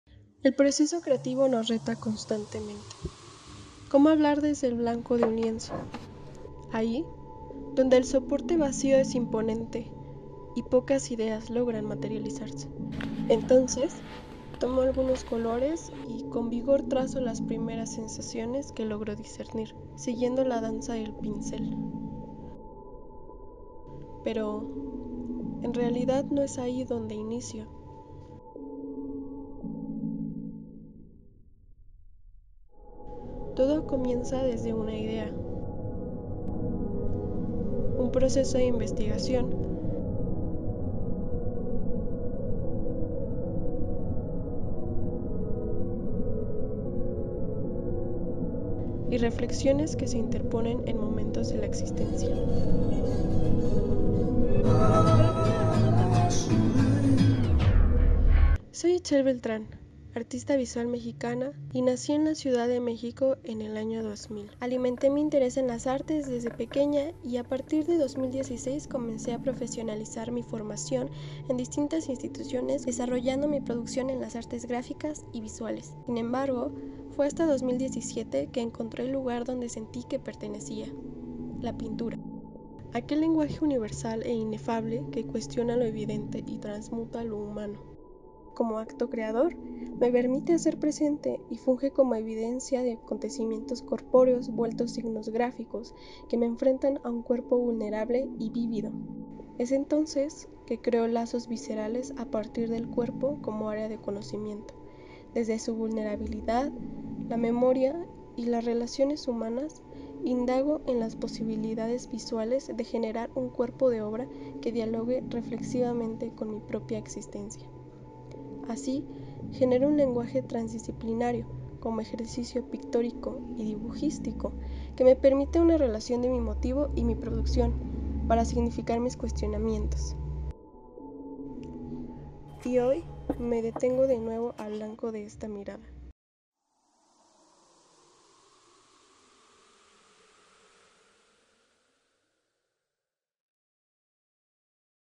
Quien soy | Itzel Beltrán | Artista Visual
Hola! Inicie este camino hace mucho pero me gustaría empezar a compartir más sobre él.

Así que les dejo este vídeo con un poco de lo que hago.